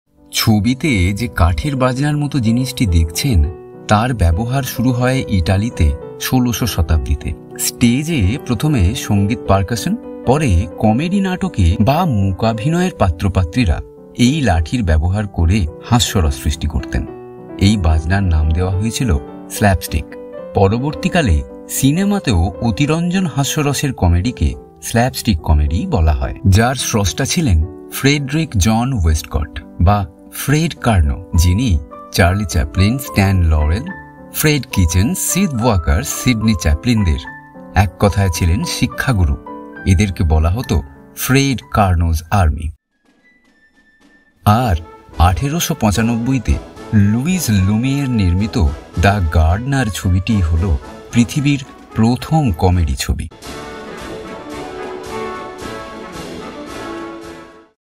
Slap Stick Comedy | Slap-Stick কমেডির ইতিহাস (History of Cinema)
Slap-Stick comedy acting was invented By Fred Karno in 1898 as a method of stage or film acting. He developed a method of acting without dialogue, sort of pantomime format but only to evoke laughter. He was the trainer of Charlie Chaplin, Stan Laurel , Sydney Chaplin and many more legendary actors.
In cinema this particular genre came into being for one and only Fred Karno. Later Charlie Chaplin, Stan Laurel made this technique into an eternal artistry.
History of cinema, history of comedy, comedy movie, inventor of comedy slapstick, what is slapstick,